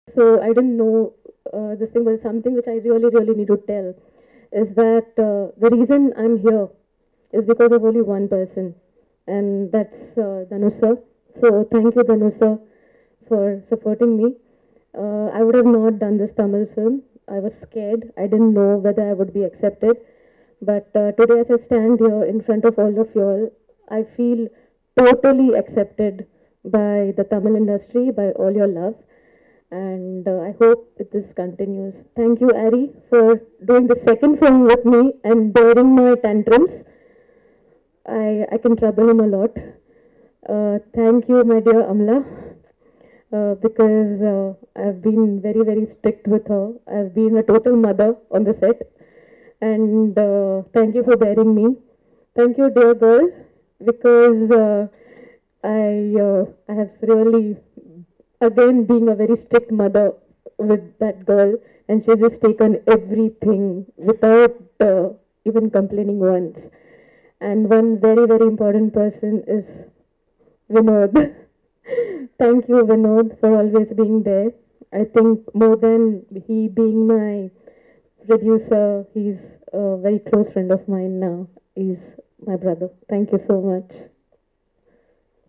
Amma Kanakku Audio Launch | Director Ashwini Iyer Tiwari
Watch Director Ashwini Iyer's speech at Amma Kanakku Tamil Movie audio launch. Amma Kanakku movie written and directed by Ashwini Iyer Tiwari. A remake of the 2016 Hindi film Nil Battey Sannata, the film stars Amala Paul, Revathi and Samuthirakani in the lead roles.

Director: Ashwini Iyer Tiwari
Producers: Anand L. Rai, Dhanush
Star Cast: Amala Paul, Revathi, Samuthirakani